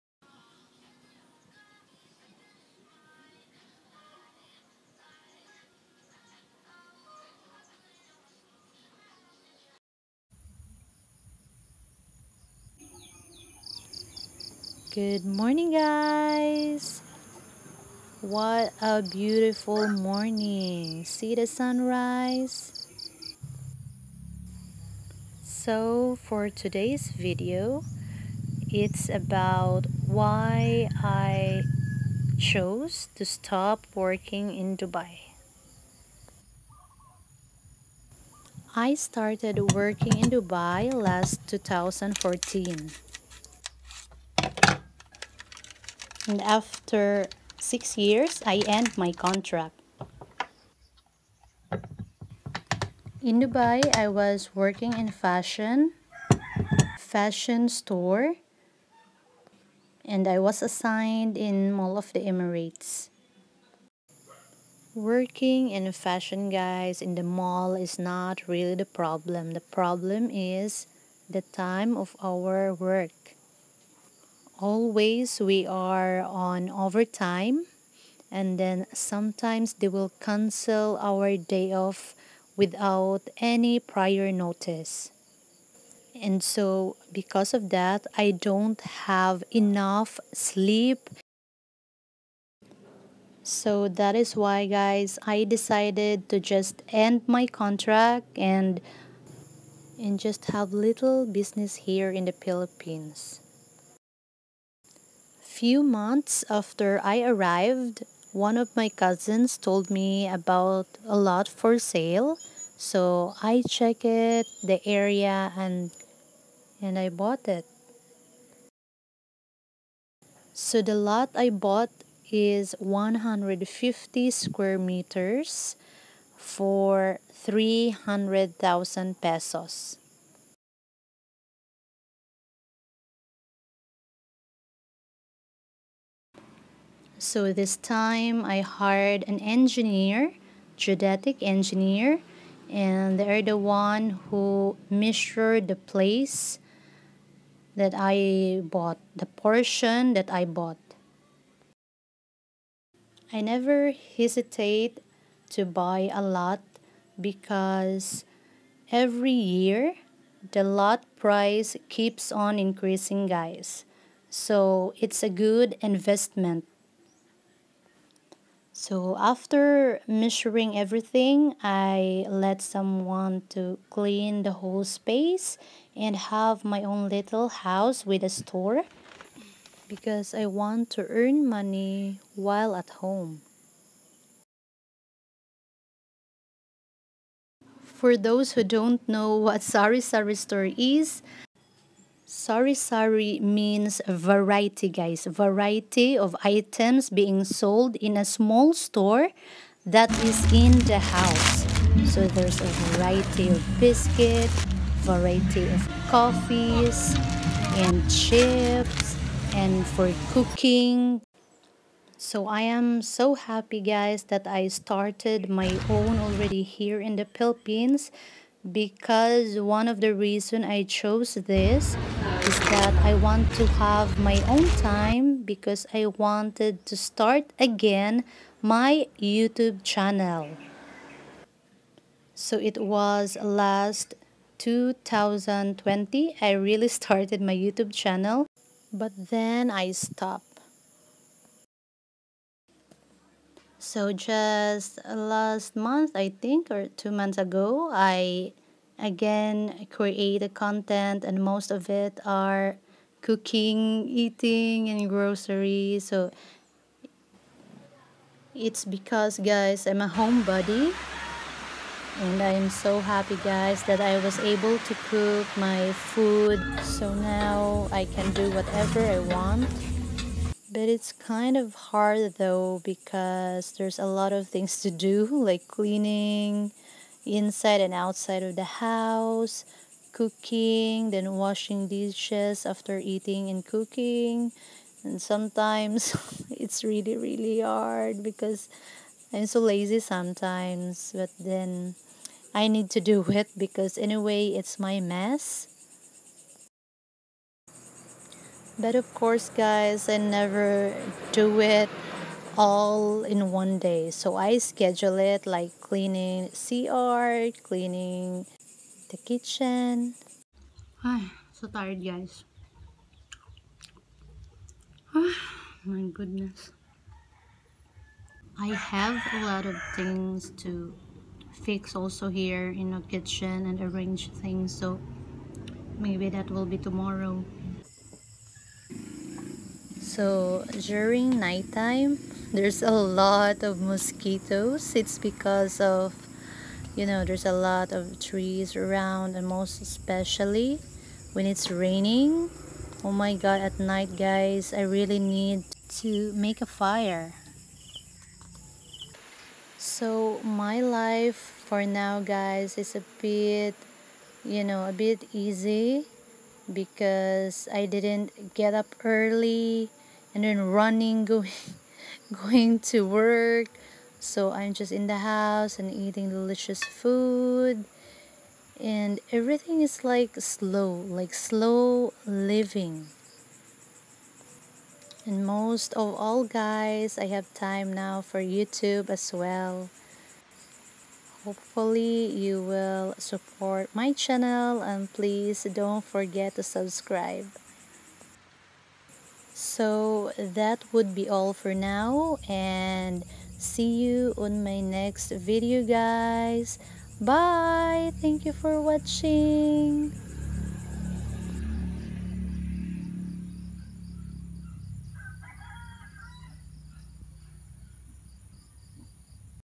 Living Alone Diaries Dubai to PH | I bought a lot in PH | RoseCeline
Hi guys! 😉😉😉

Most of my videos is about lifestyle, cooking, eating & having groceries.

Living alone life in dubai is my content in this video. I end my contract in Dubai for some reasons and chose to stop working and have my little business here in the Philippines 🇵🇭.